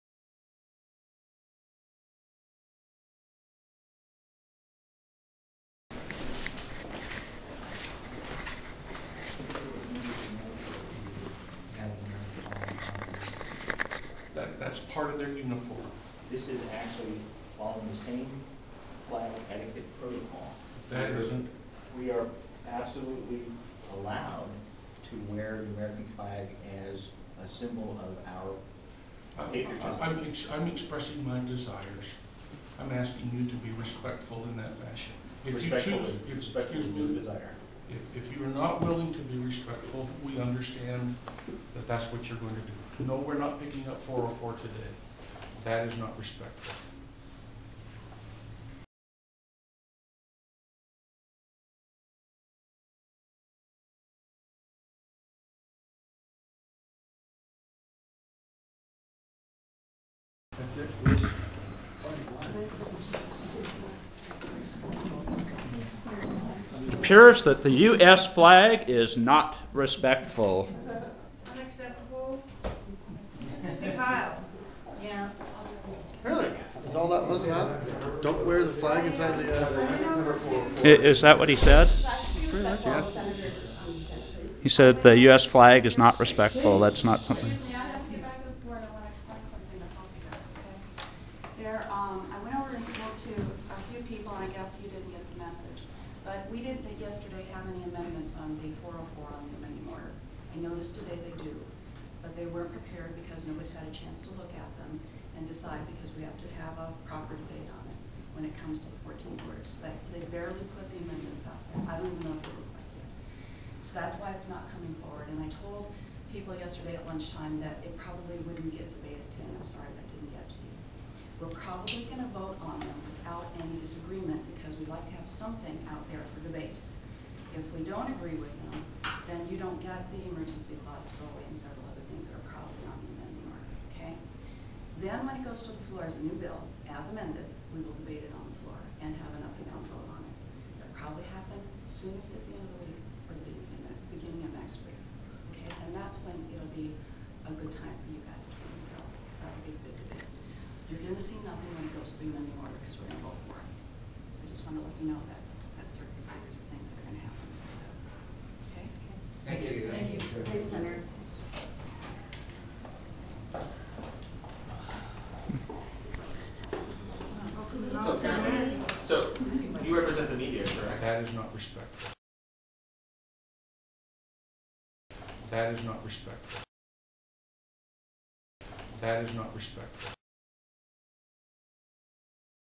Idaho State Senator Bart Davis says wearing US Flag is not respectful.wmv
Occupy Boise members are removed from the Senate Gallery just as the hearing for HB 404, the anti-free speech bill is about to get underway. Senator Bart Davis, the Majority Leader, gives them a stern lecture about wearing the American Flag. He says "That is not respectful".

No first amendment rights for you! No wearing the American Flag!